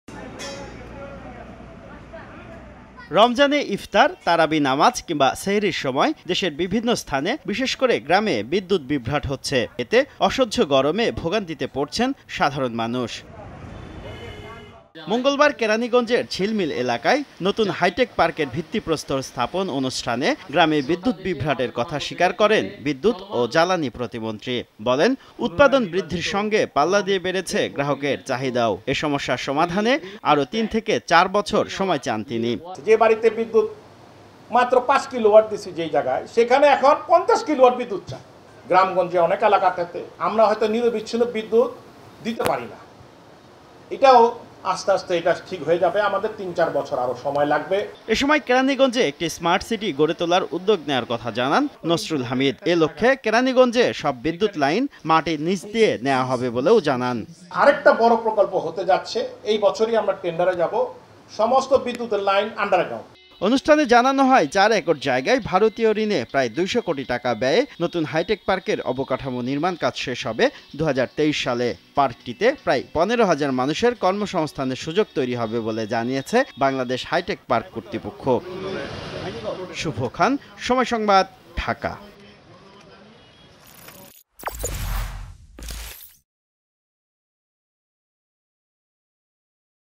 'নিরবচ্ছিন্ন বিদ্যুৎ-গ্যাস সরবরাহে ঐক্যবদ্ধ প্রচেষ্টা প্রয়োজন' | Electricity | Nasrul Hamid
আগামী তিন-চার বছর পর গ্রামে আর বিদ্যুৎ বিভ্রাট থাকবে না বলে জানিয়েছেন,বিদ্যু ও জ্বালানি প্রতিমন্ত্রী নসরুল হামিদ। গ্রামে নিরবচ্ছিন্ন বিদ্যুৎ সরবরাহে এখন থেকে প্রয়োজনীয় পদক্ষেপ নেয়া হচ্ছে বলে জানান তিনি। মঙ্গলবার সকালে কেরানিগঞ্জে এক অনুষ্ঠানে "স্মার্ট সিটি" গড়তে কেরানিগঞ্জে সব বিদ্যুৎ লাইন মাটির নিচ দিয়ে নেয়ার উদ্যোগ নেয়া হয়েছে বলেও জানান নসরুল হামিদ।